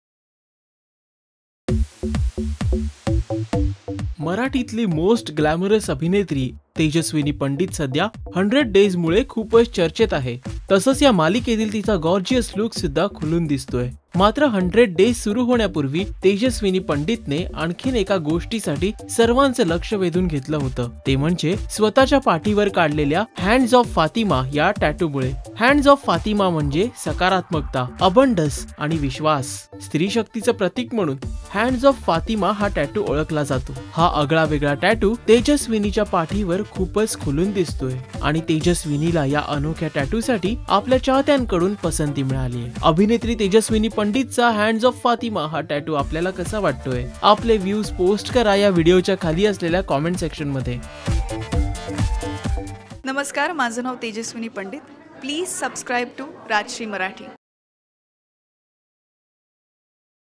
Tejaswini Pandit Gets A Unique Tattoo | 100 Days | New Serial on Zee Marathi
100 Days serial fame actress Tejaswini Pandit get inked! Check out her unique tattoo only on Rajshri Marathi!